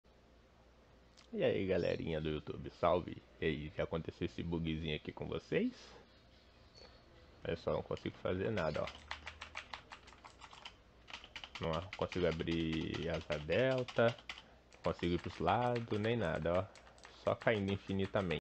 O JOGO BUGOU E FIQUEI CAINDO INFINITAMENTE KKKKKKKKKKKK
☑️   APOIE NA LOJA DA EPIC GAMES : EMERSONVF  ☑️

❤️‍🔥   CANAL DE PEQUENOS CORTES DE LIVES  ❤️‍🔥